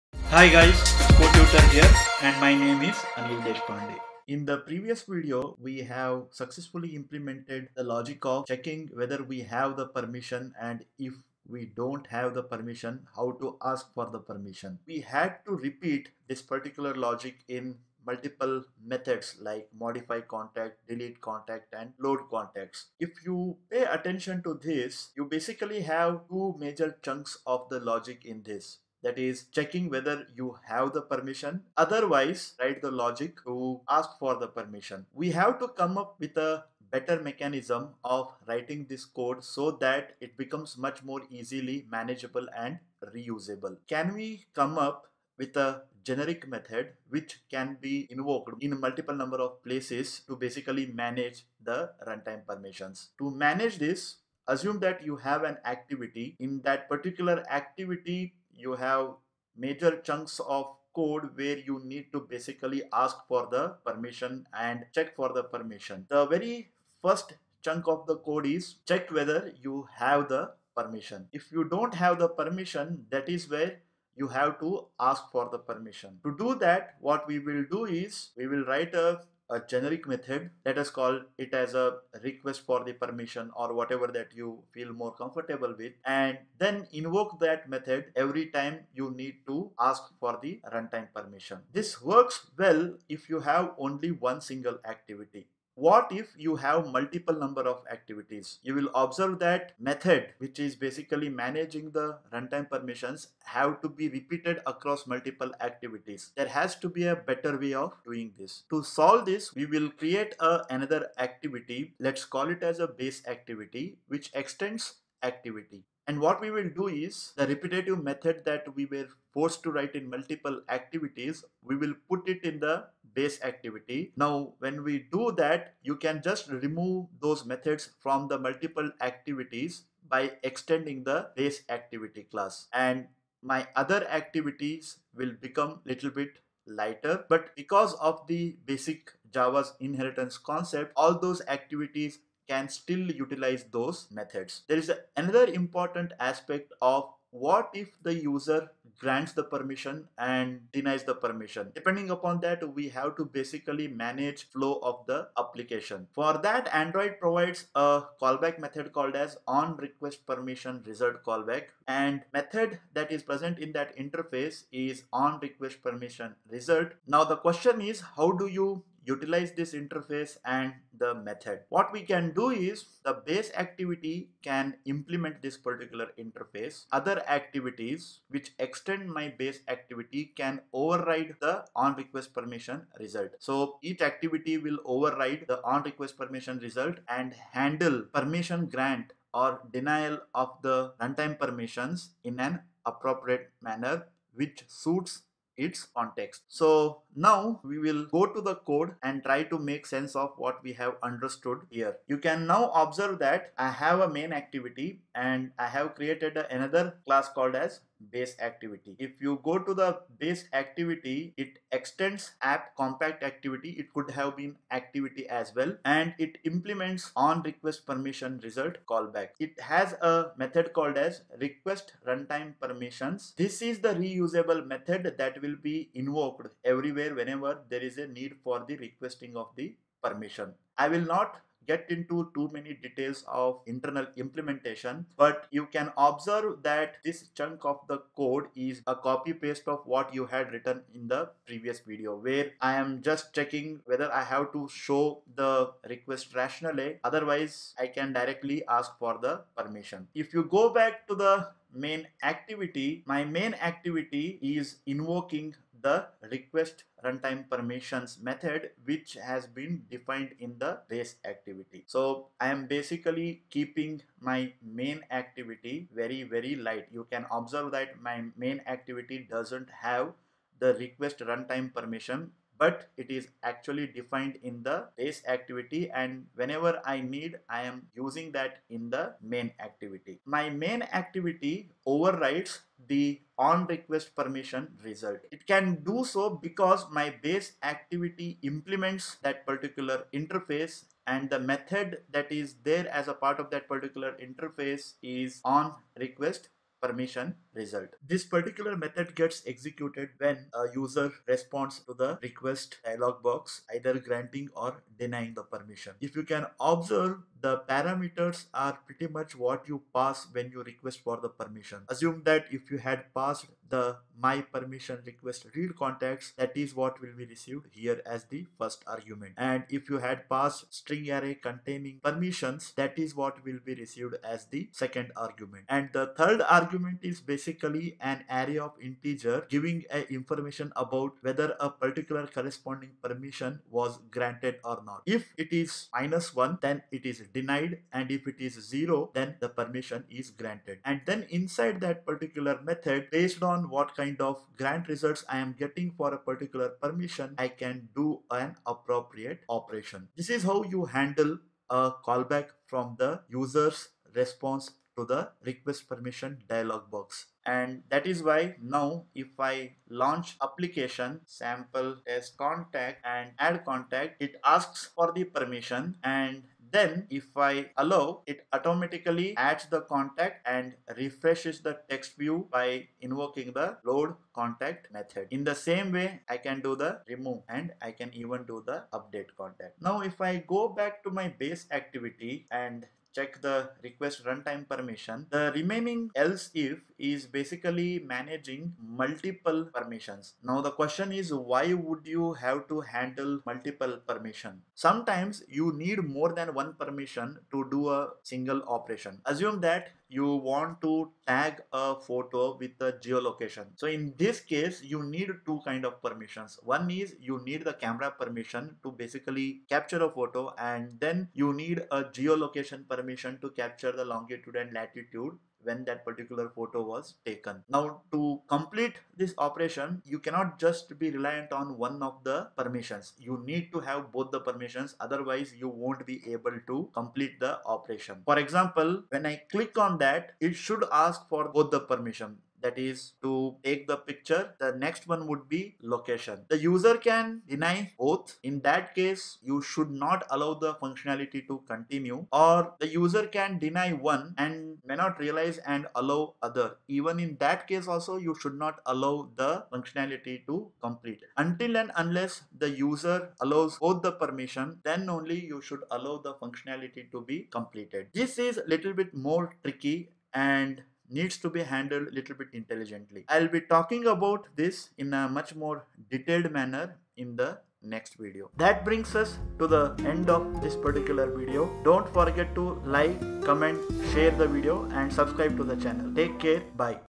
Android Permissions - Part 4, Design pattern for managing Runtime permissions
It is quite possible that we need to ask for permissions in an App, from multiple activities and also handle user response from multiple Activities. In this video we explore how to come up with such a design pattern so that run time permission handling becomes a reusable component across the app components. The video then goes through the code showing the implementation details through code.